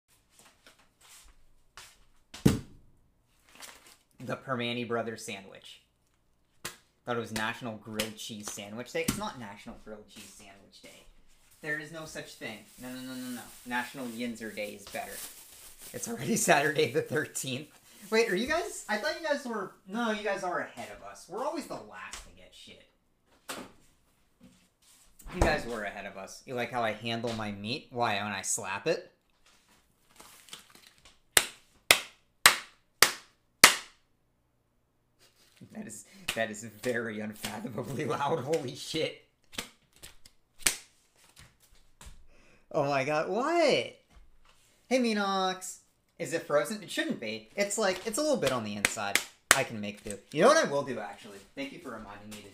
Maddie slaps her meat
This is a clip from FemboyDrumming's Twitch stream entitled BUNNY SUIT COOKING STWEAM 🐰| YLYL AFTER? | !S Socials 🥵 which originally streamed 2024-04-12.

This is a fan-made and uploaded video. FemboyDrumming retains all rights to the stream content.